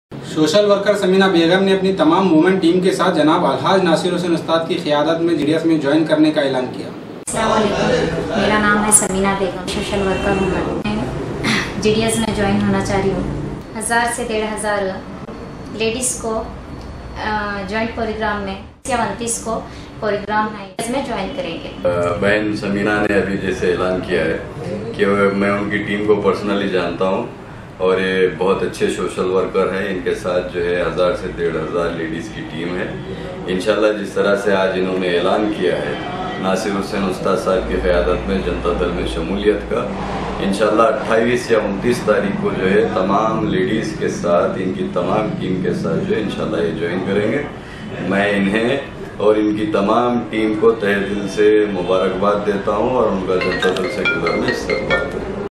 Social Werker Sameena Begum Ne Apne Women Team Ke saat JDs Join karne ka Elaan Kiya.....
Social Werker Sameena Begum Ne Apne Women Team Ke saat Janab Nasir Hussain Ustad Ki Khiyadat Me JDs Join karne ka Elaan Kiya.